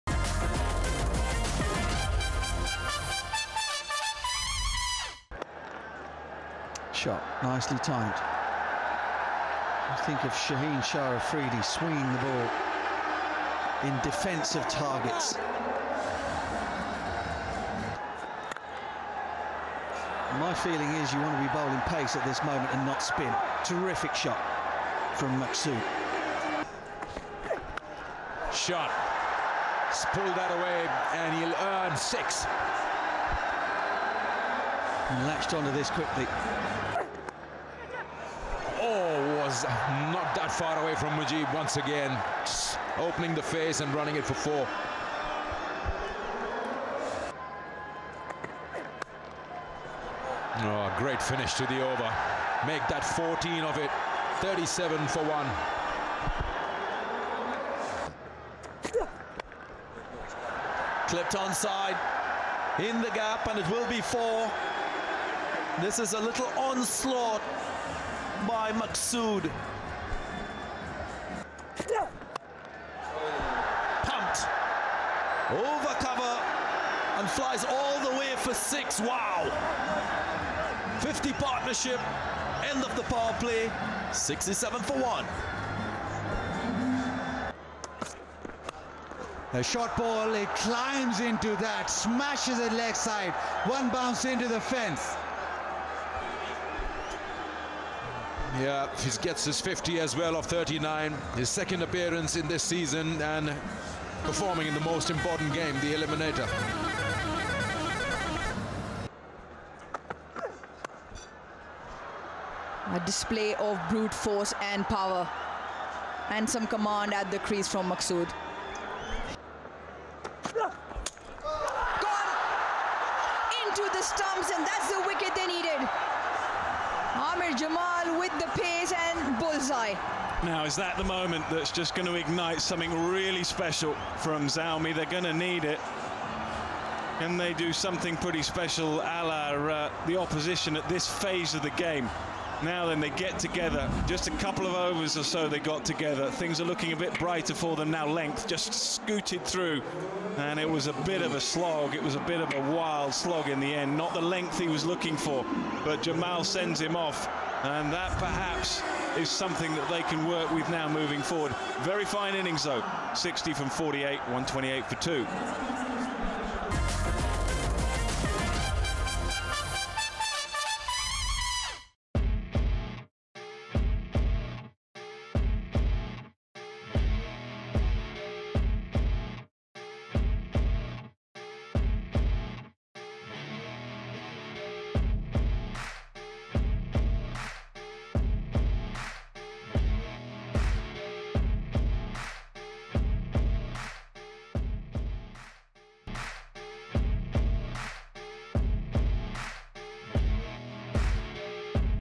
Sohaib Maqsood 60 runs vs Peshawar Zalmi| Eliminator 1 - Islamabad United vs Peshawar Zalmi
Sohaib Maqsood 60 runs vs Peshawar Zalmi| Islamabad United vs Peshawar Zalmi| Eliminator 1 - Islamabad United vs Peshawar Zalmi
📺: Willow TV